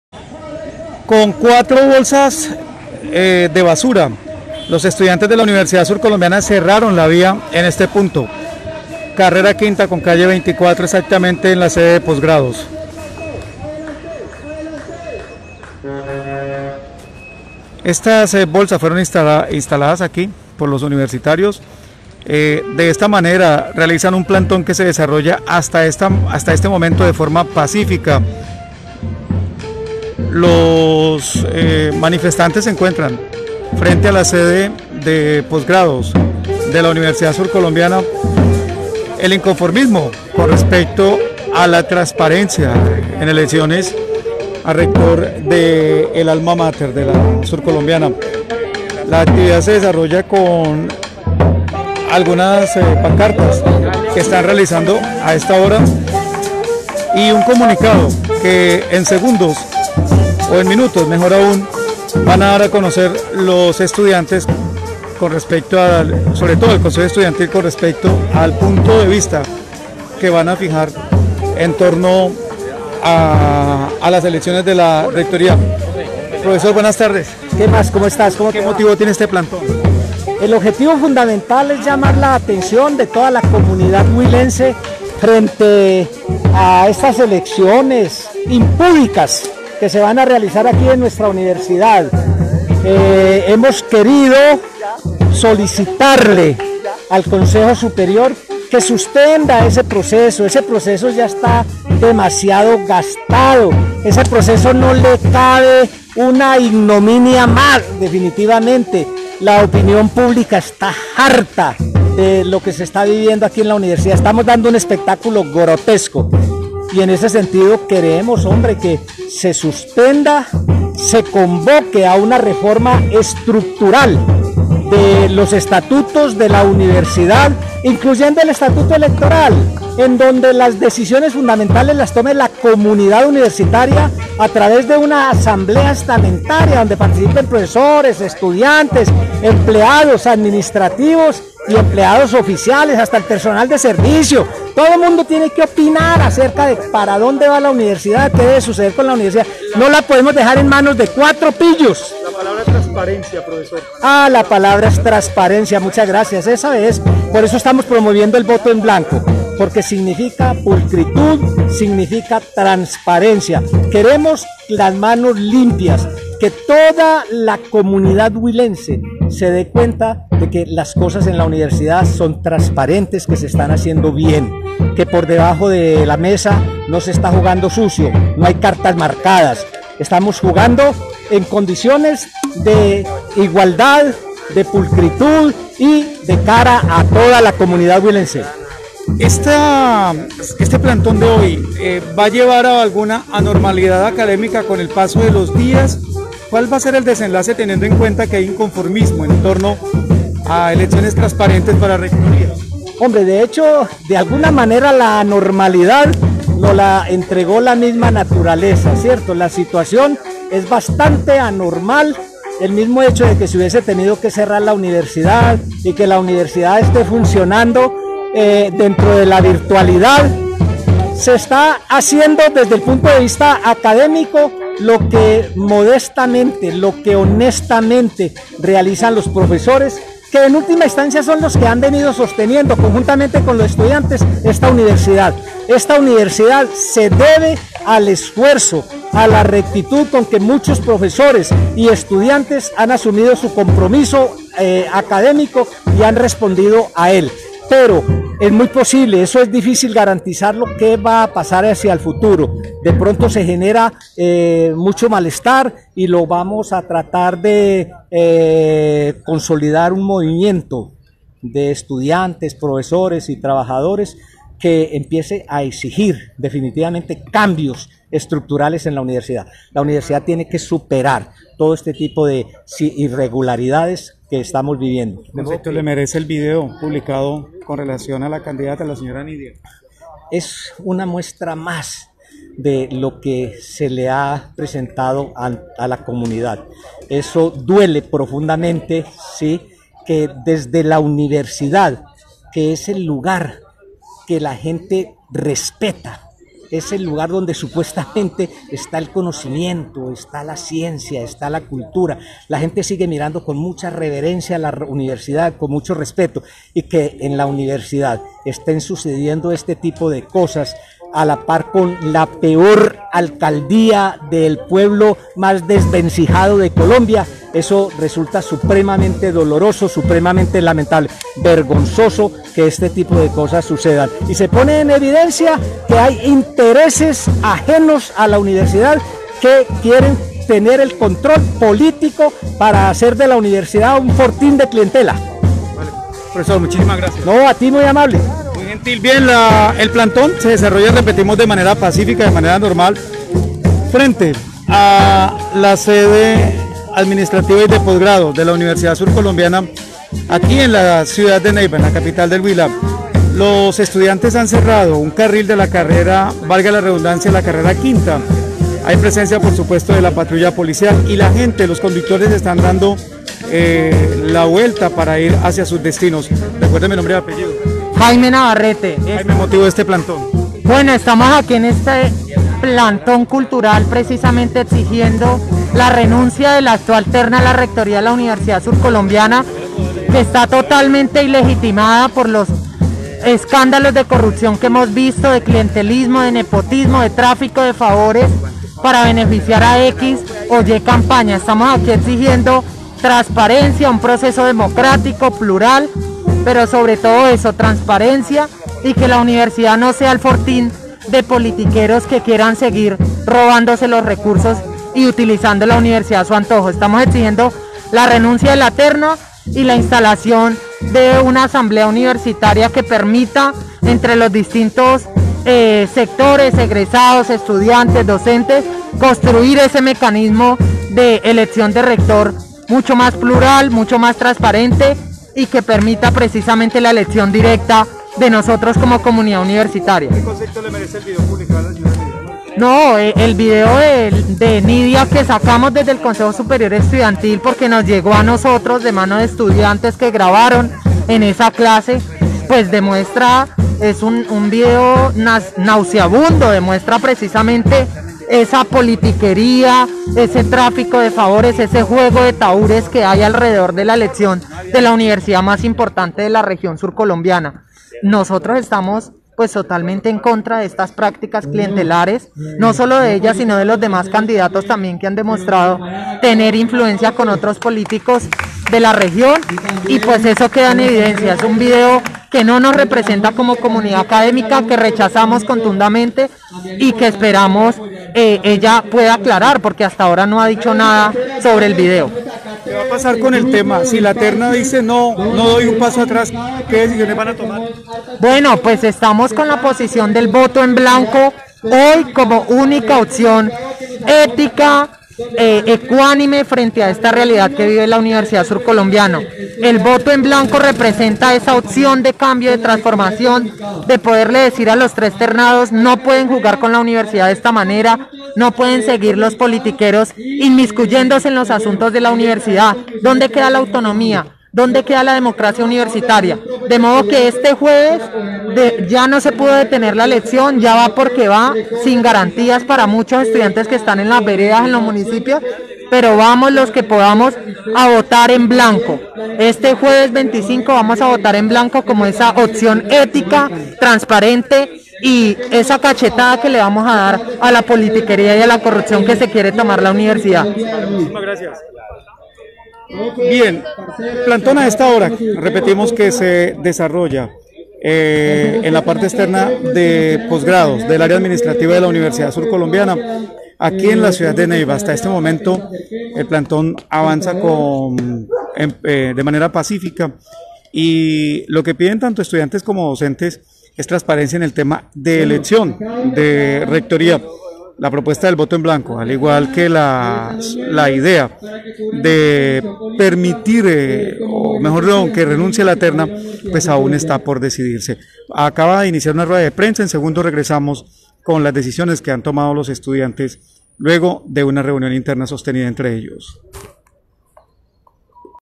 Plantón y cierre de vía a esta hora por elecciones transparentes a rectoría de la USCO
Plantón y cierre de vía a esta hora por elecciones transparentes a rectoría de la Universidad Surcolombiana